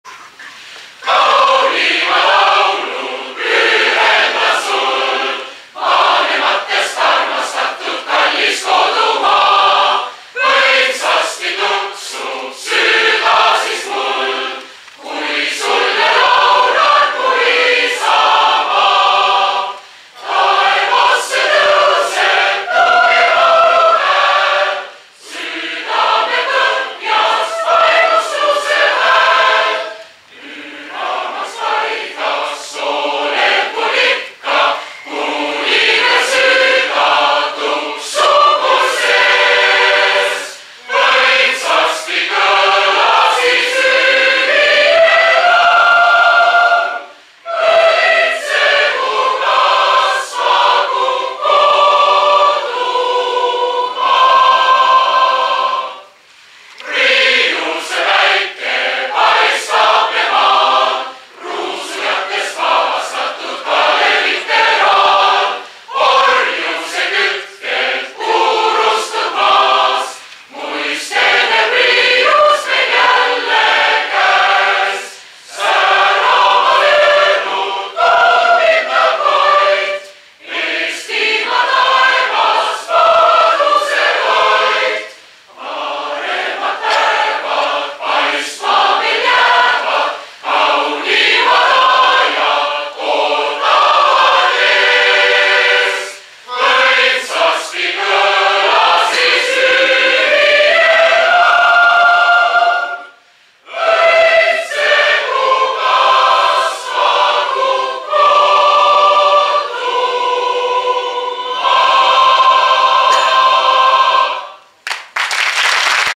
Seedrioru VI - Kaunimad laulud
Estonian language song festival held at Seedrioru summer camp near Elora Ontario Canada, June 28 2008.